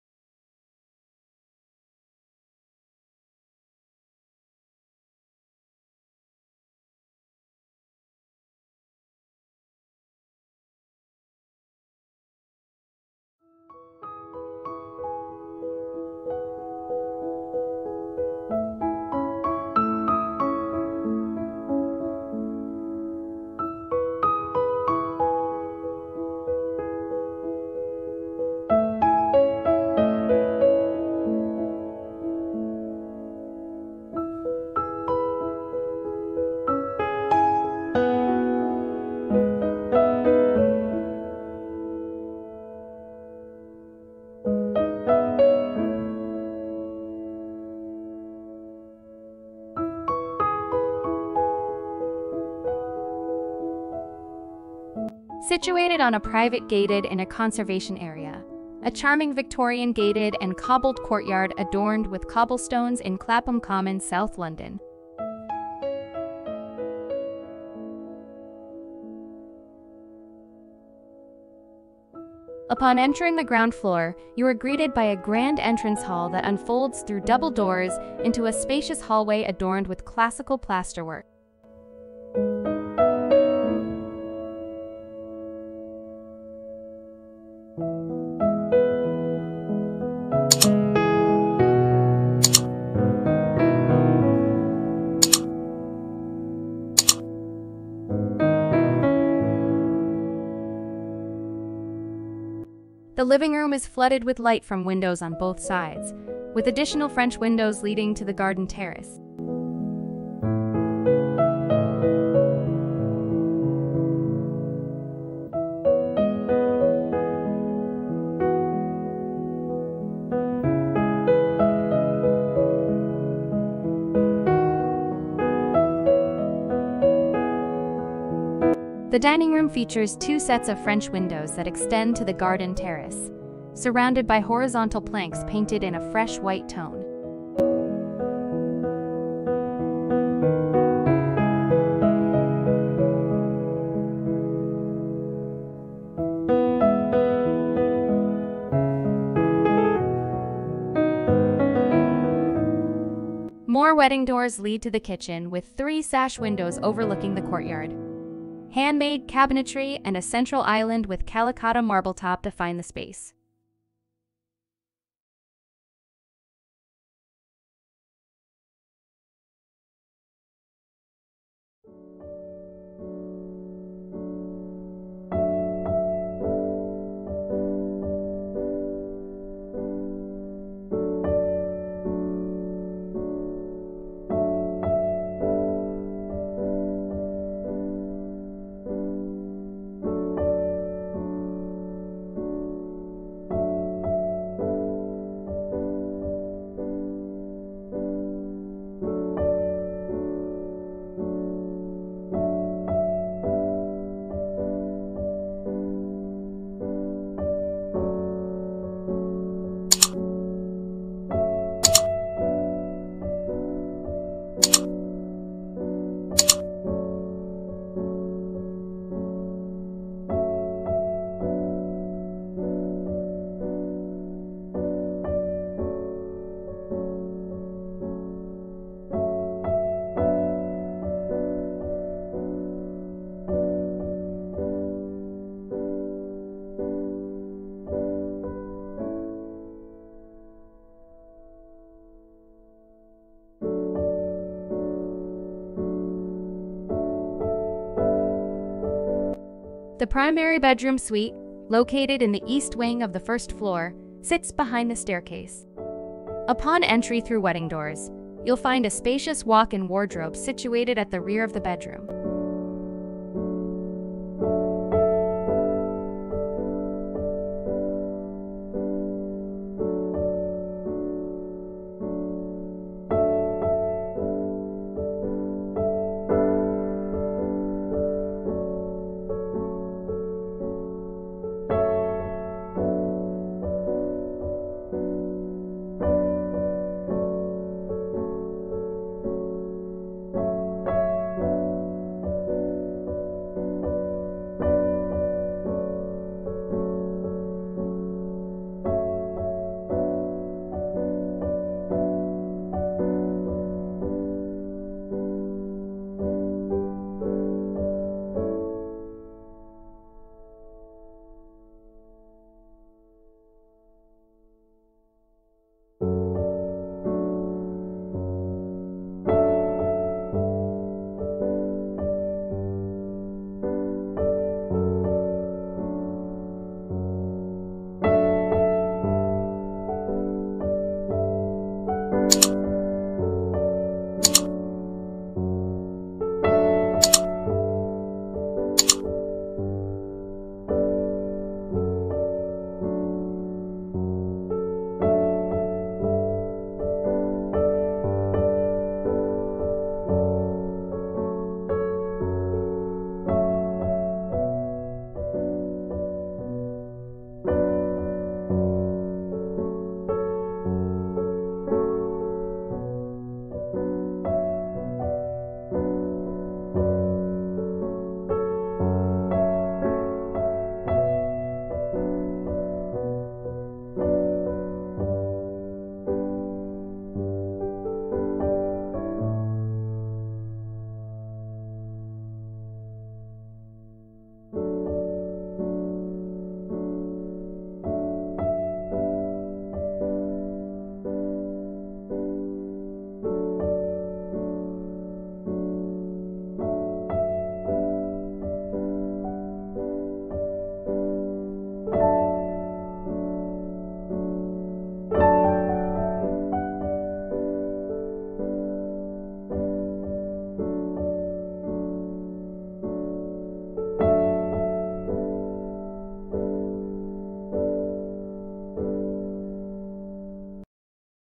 Inside A Historic Home That Features Elegant Design Elements
Located between Clapham Common and Battersea Park, within the Wandsworth Road conservation area, lies Turnchapel Mews—a delightful Victorian gated courtyard adorned with cobblestones. This historic enclave, once graced by the presence of acclaimed author Roald Dahl and British artist Euan Uglow, offers a picturesque setting off Cedars Road.